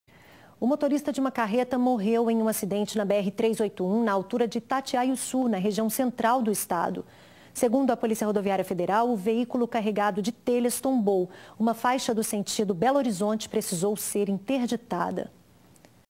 Carreta tomba e mata motorista
O veículo transportava telhas e tombou, ocasionando a morte do condutor. Uma faixa da BR-381 precisou ser interditada.